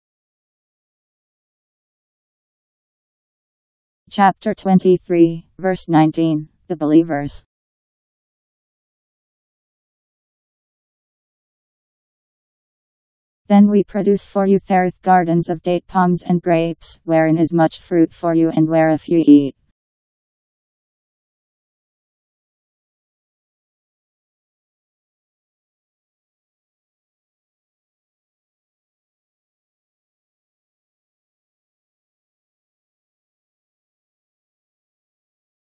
Quran The Believers 23:19
Video shows Quran The Believers Chapter 23 Verse 19: Then We produce for you therewith gardens of date-palms and grapes, wherein is much fruit for you and whereof ye eat; Powered by MaryTTS. Holy Qur'an 23:19 Surah : Ayah